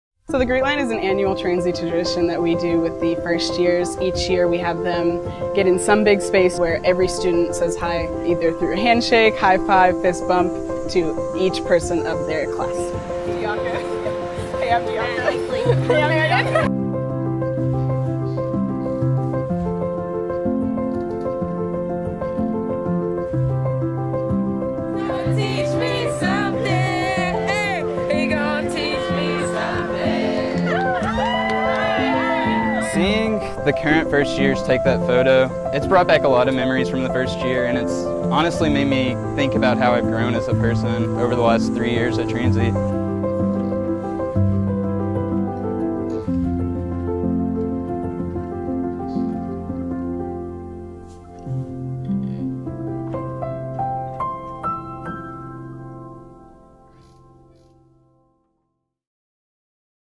Transylvania Traditions - Greet Line
Each incoming Transylvania class greets each other to wrap up their first weekend on campus.